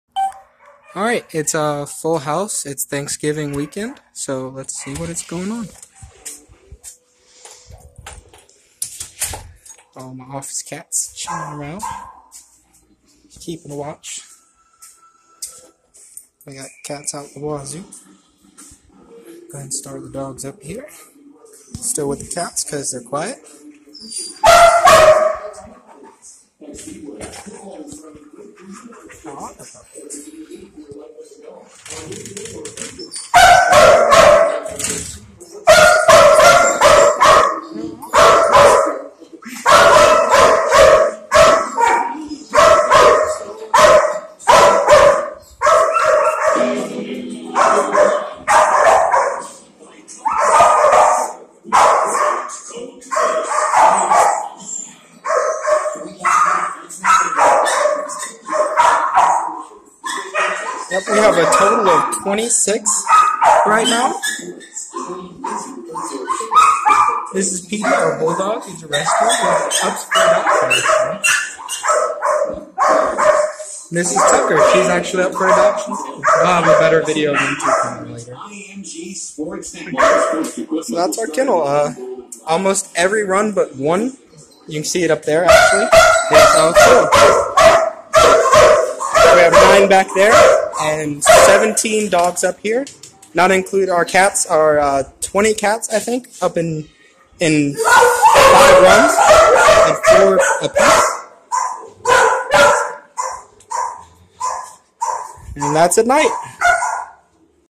Full Thanksgiving Kennel(Loud)
We have 26 doges currently staying with us, a big thank-you  to Kevin Rang for subbing :D